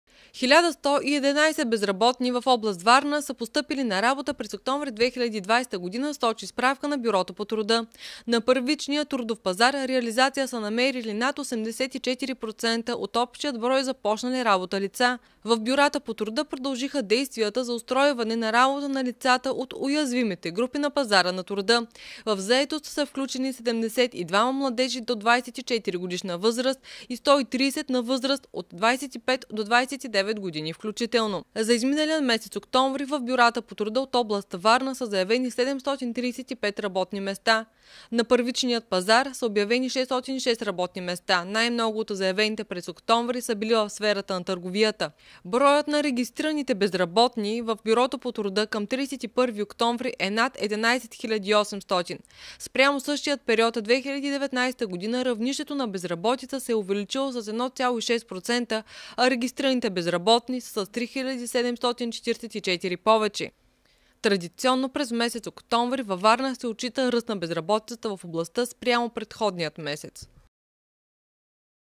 Над 1000 лица са постъпили на работа във Варна през октомври
1111 безработни в област Варна са постъпили на работа през октомври 2020г., сочи справка на бюрото по труда. . На първичния трудов пазар реализация са намерили над 84% от общия брой започнали работа лица. В бюрата по труда продължиха действията за устройване на работа на лицата от уязвимите групи на пазара на труда. В заетост са включени 72 младежи до 24 г. възраст и 130 на възраст от 25 до 29 г. включително. Преходите в заетост на млади хора са 18,% от всички реализирани през месеца преходи в заетост. През октомври 2020 г. на работа са устроени и 82 продължително безработни лица (с регистрация в бюрата по труда над 1 година).

За миналия месец месец. в бюрата по труда от област Варна са заявени 735 работни места. На първичния пазар са обявени 606 работни места. Най-много от заявените през октомври са били в сферата на търговията . Следват хотелиерство и ресторантьорство (74), преработваща промишленост (73), образованието (72) и др.

Броят на регистрираните безработни в бюрата по труда от областта към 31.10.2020 г. е над 11 800. Спрямо същия период 2019 г. равнището на безработица се е увеличило с 1,6%, а регистрираните безработни са с 3744 повече. Традиционно през месец октомври се отчита ръст на безработицата в областта спрямо предходния месец.